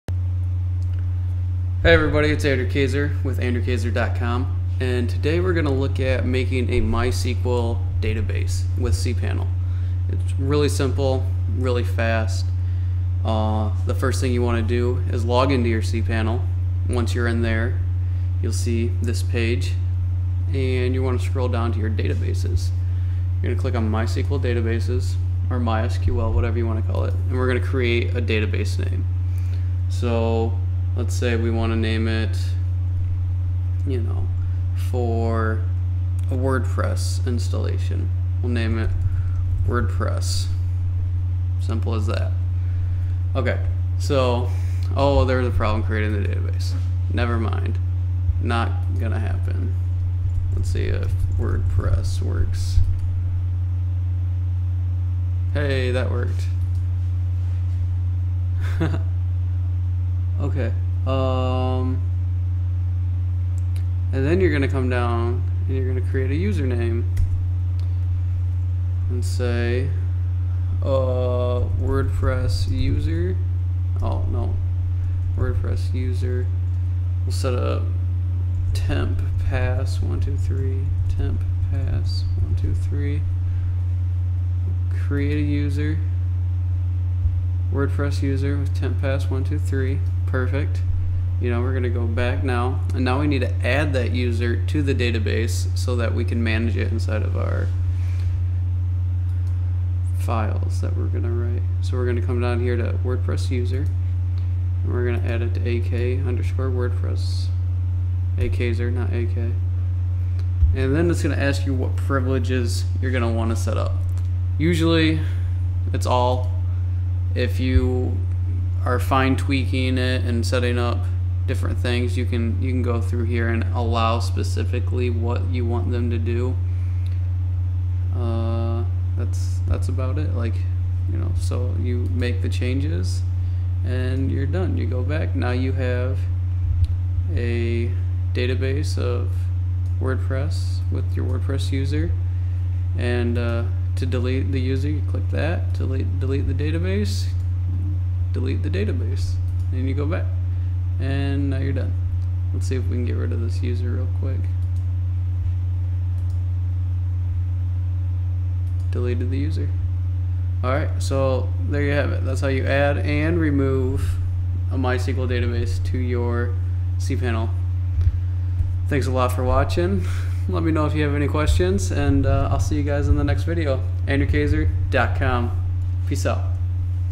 Create A MySQL Database with cPanel - EASY tutorial
How To Create a MySQL database with cPanel
In this video I show you how quick and easy it is to create a MySQL database inside of cPanel and how to create and add a user to the database.

Want to show some support?